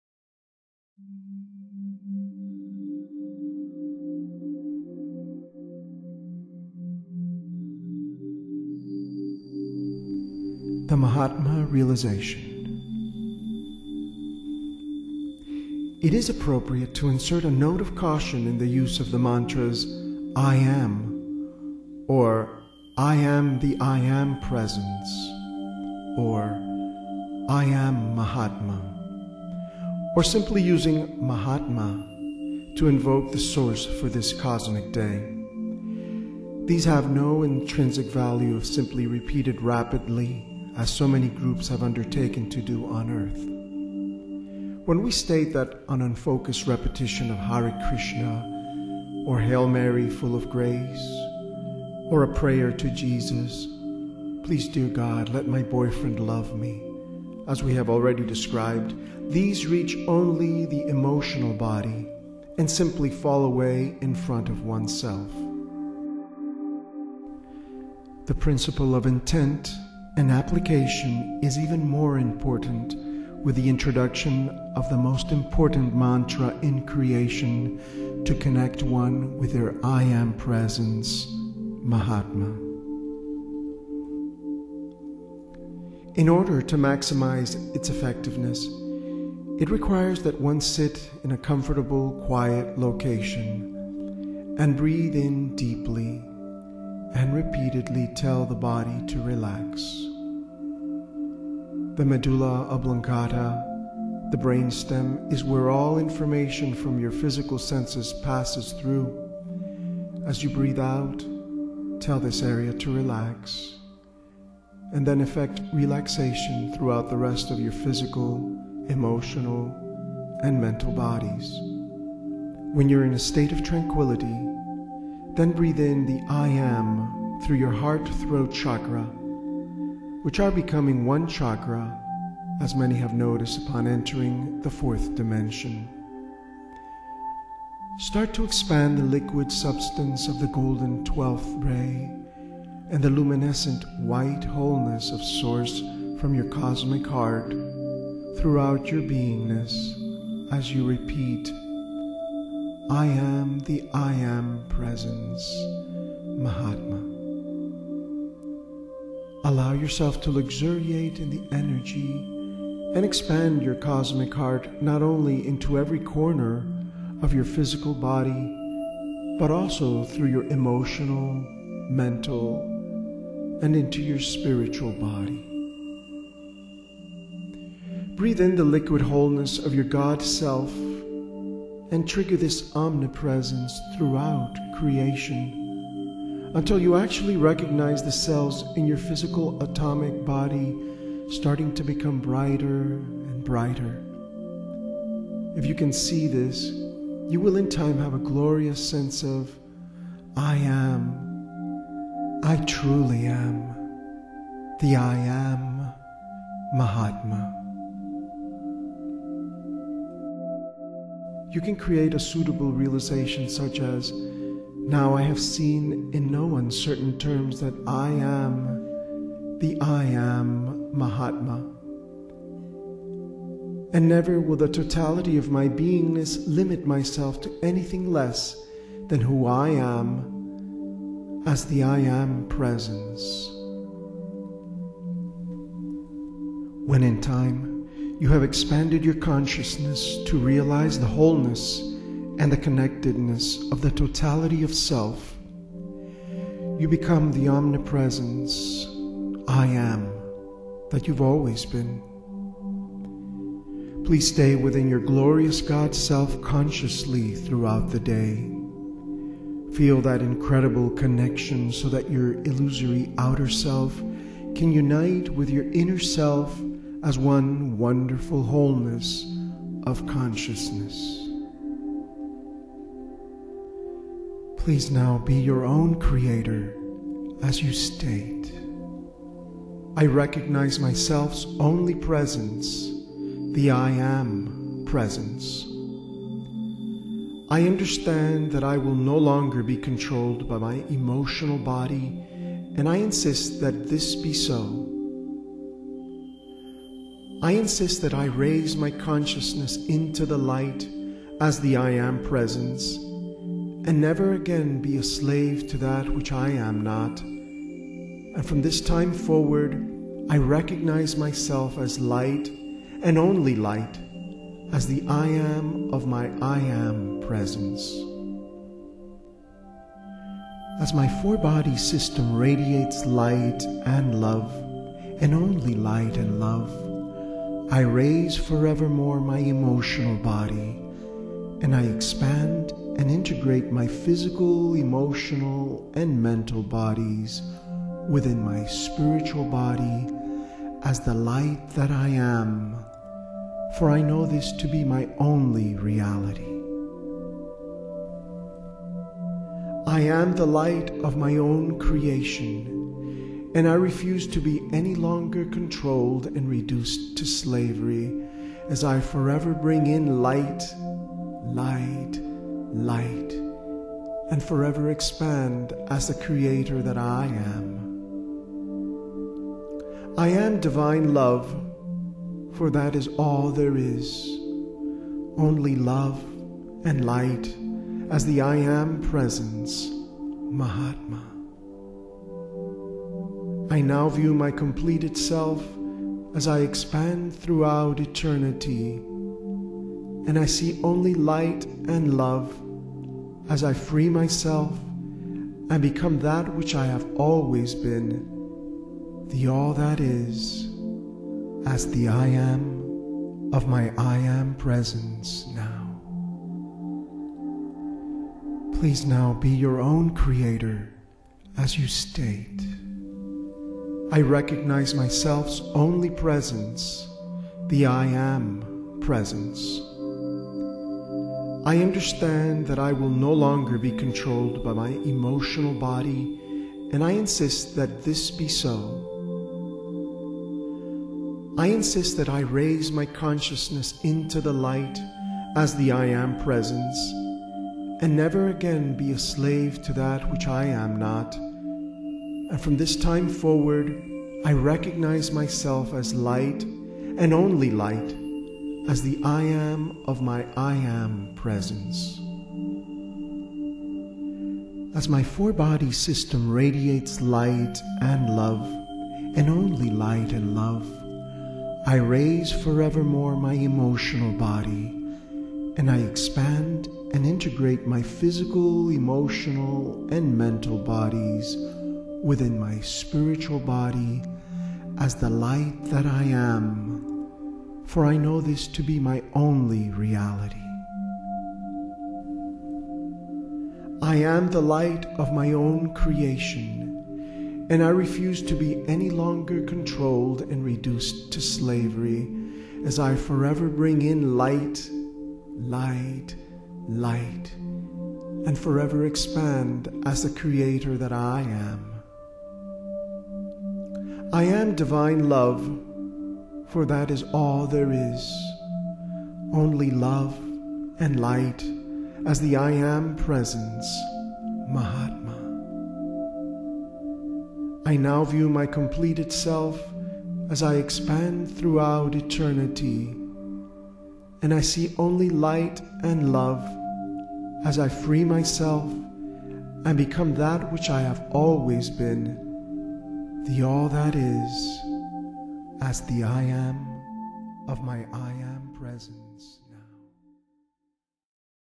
Mahatma Realization
These words and affirmations are simple and yet immensely profound. Follow these and and begin your journey, allowing all levels of self to be transformed into your whole and unlimited higher self.